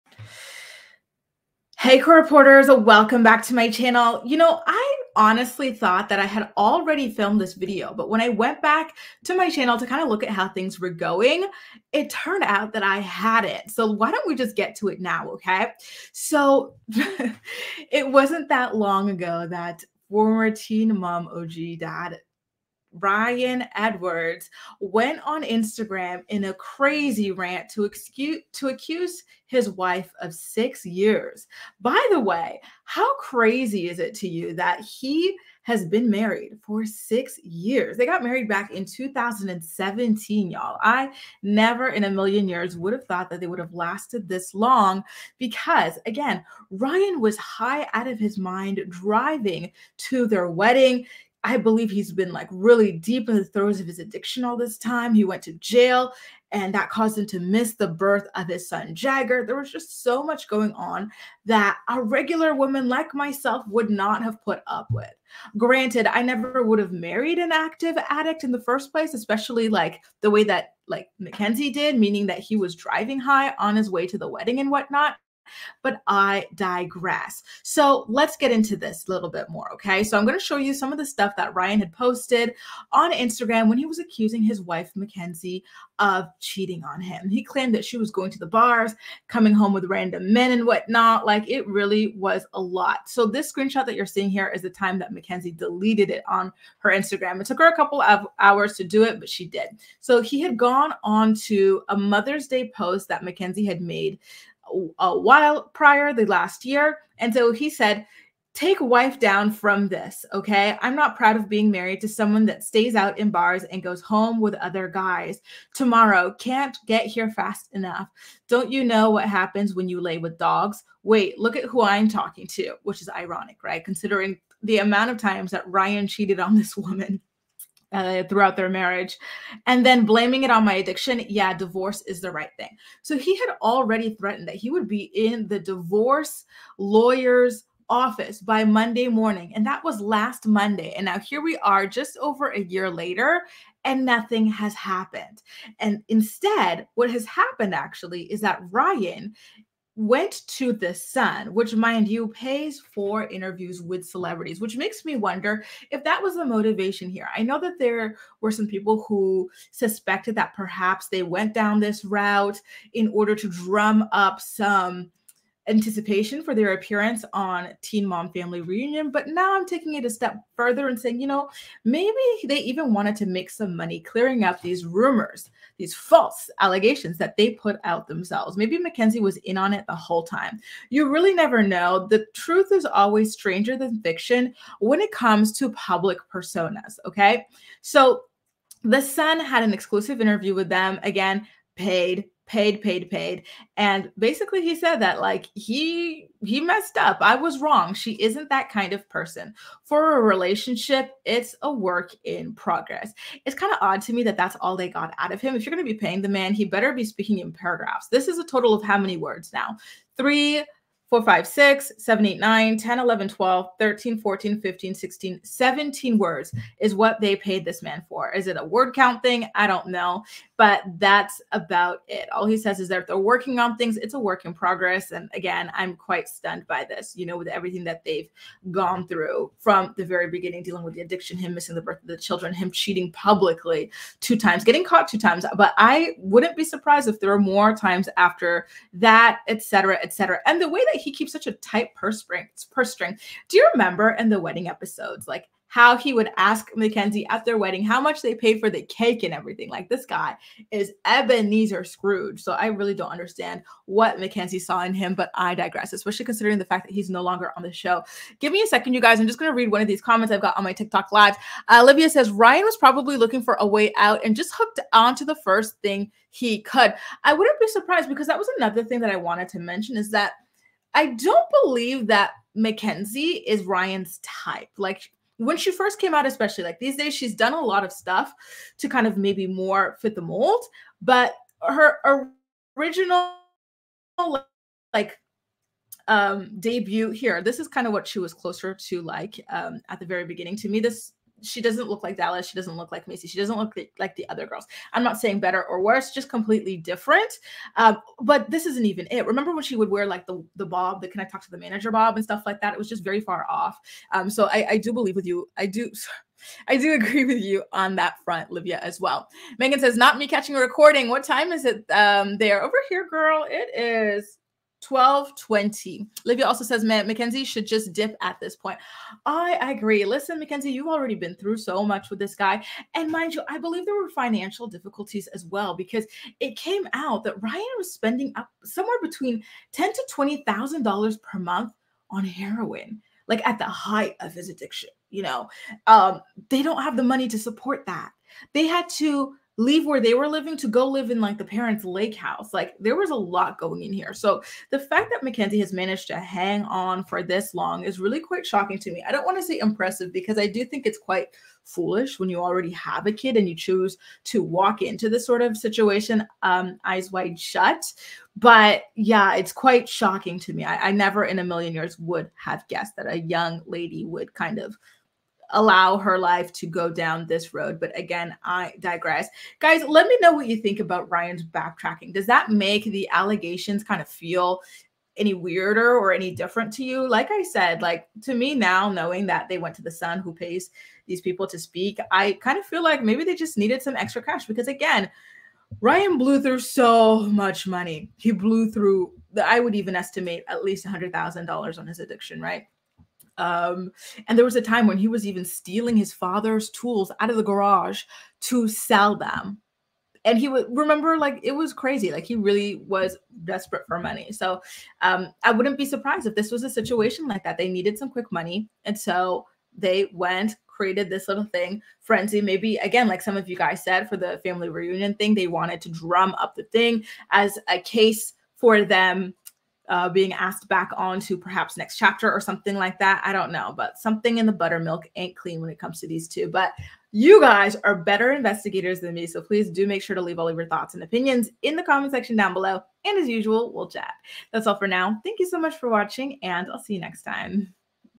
Ryan Edwards BACKTRACKS on Cheating Allegations!
Former Teen Mom: OG star Ryan Edwards is now backtracking on the cheating allegations he made against his wife Mackenzie Standifer.